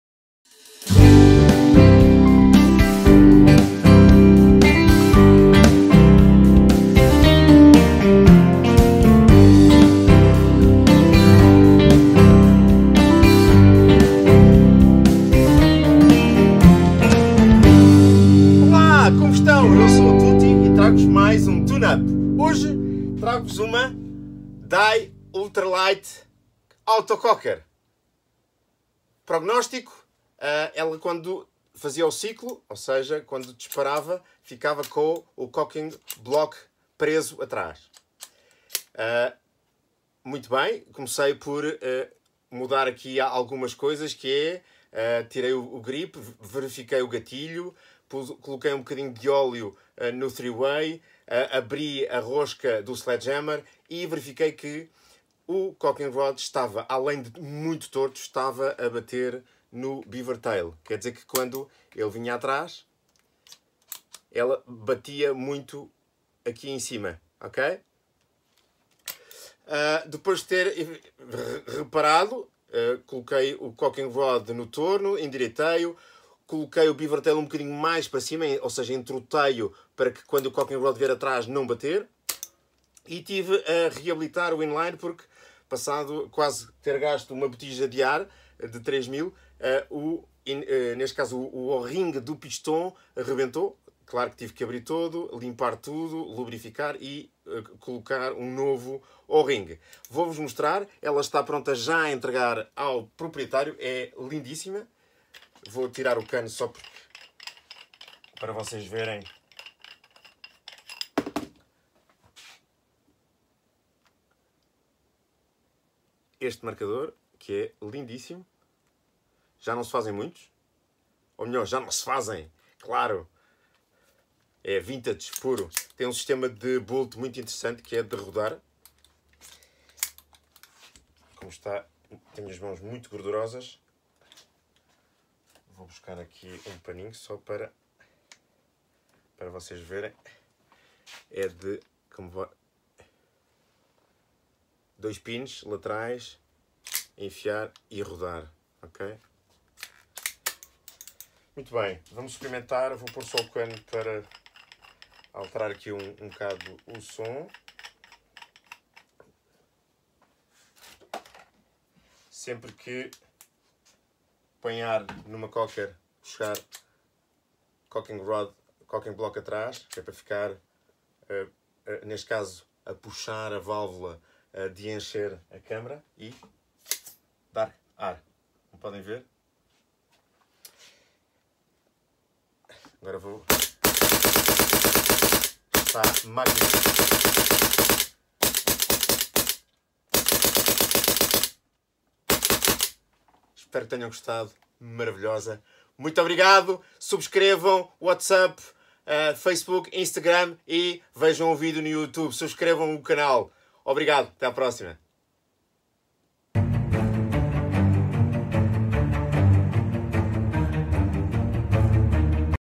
64# AUTOCOCKER DYE REFLEX TUNE UP _byTutty
TUNE UP

-Ficava com o cocking-block preso=gatilho ficava preso
-Cocking ROD empenado
-Reabilitação do HP in line regulator
-Alinhamento beavertail

Lubrificação:
-Bolt
-Gatilho
-3Way

Para mais informações:
Portugal